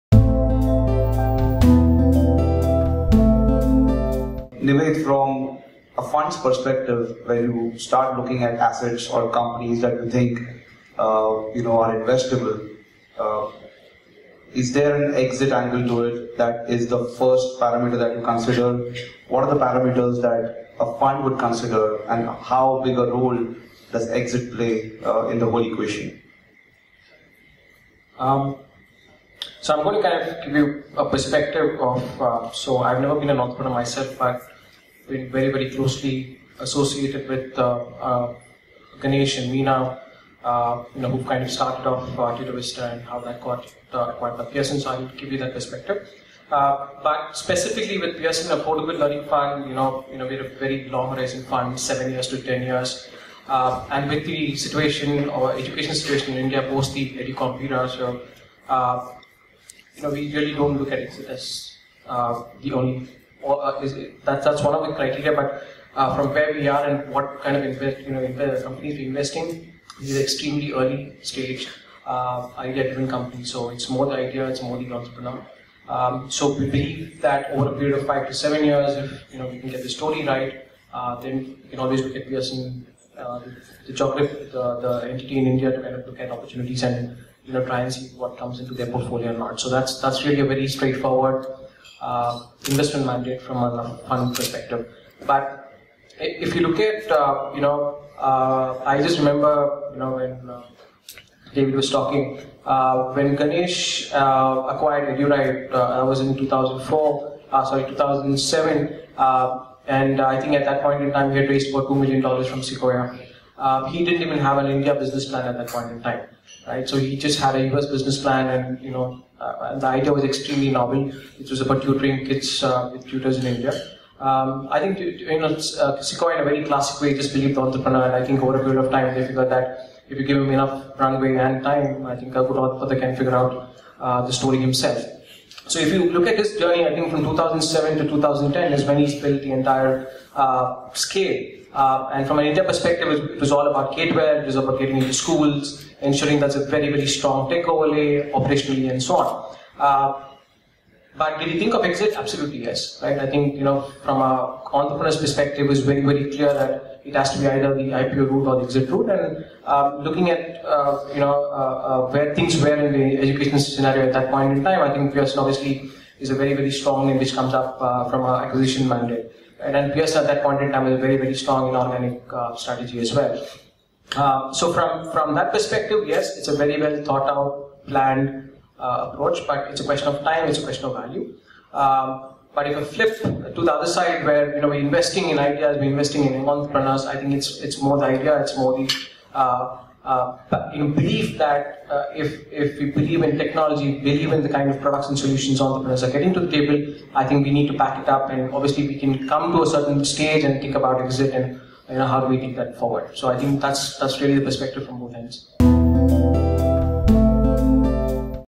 Startup2Startup Mumbai 2015: Fund’s perspective on Exit
Nived Dinesh from Pearson Affordable Learning Fund shares his experience of a Fund's eye on an eventual exit while investing in any venture.